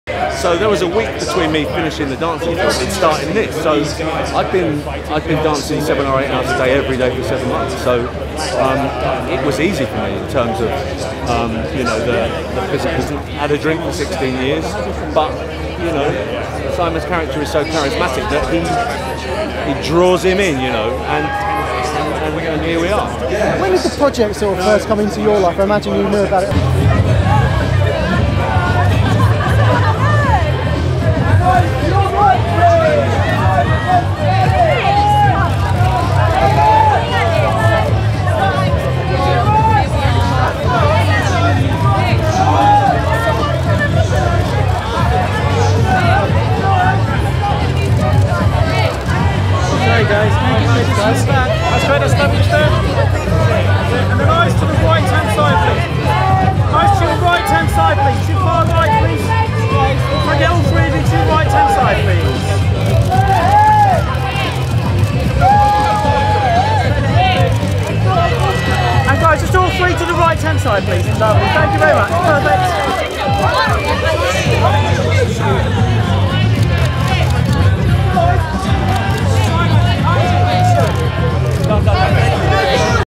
B-ROLL - Simon Pegg, Nick Frost, Rosamund Pike at 'The Wo...
B-ROLL - Simon Pegg, Nick Frost, Rosamund Pike at 'The World's End' World Premiere at Empire Leicester Square on July 10, 2013 in London, England.